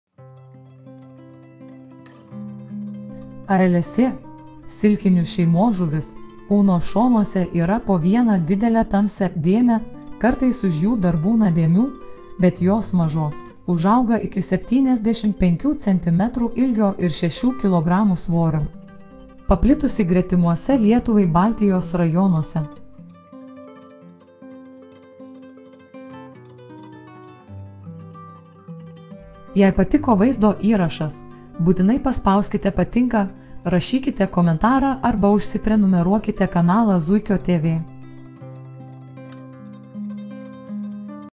Alsė
Alsė – silkinių šeimos žuvis. Kūno šonuose yra po vieną didelę tamsią dėmę, kartais už jų dar būna dėmių, bet jos mažos. Užauga iki septyniasdešimt penkių centimetrų ilgio ir šešių kilogramų svorio. Paplitusi gretimuose Lietuvai Baltijos rajonuose.